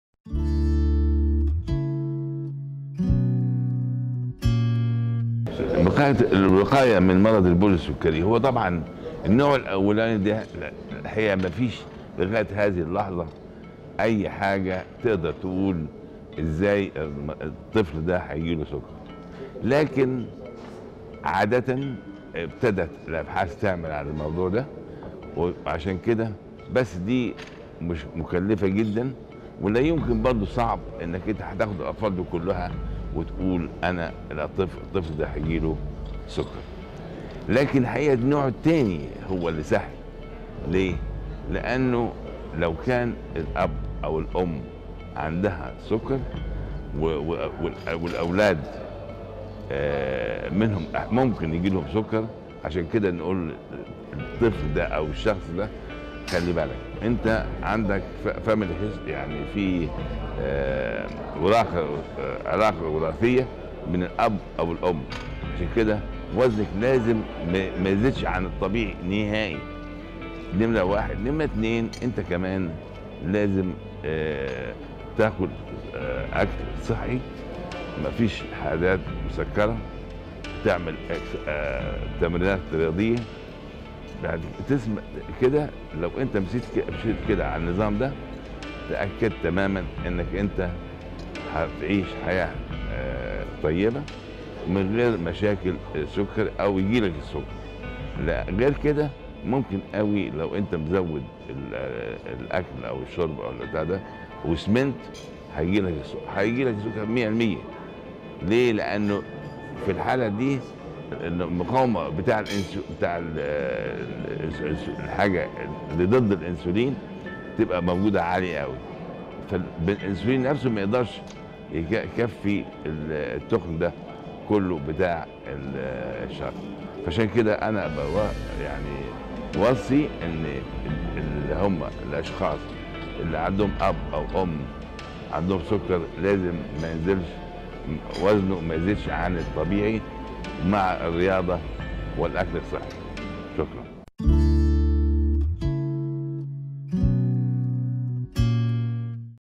طرق الوقاية من مرض البول السكرى / دكتور مغازى على محجوب
الدكتور مغازى على محجوب
أستاذ السكر و الغدد الصماء كلية طب جامعة عين شمس